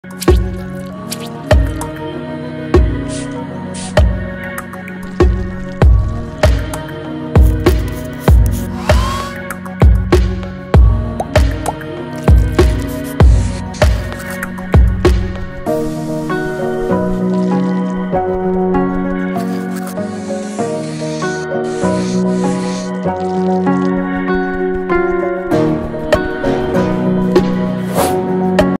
Tree-shaped sponge painting of spring scenery?
How to paint a spring scene with a tree-shaped sponge? | painting for beginners | easy painting | painting ideas | canvas painting | painting easy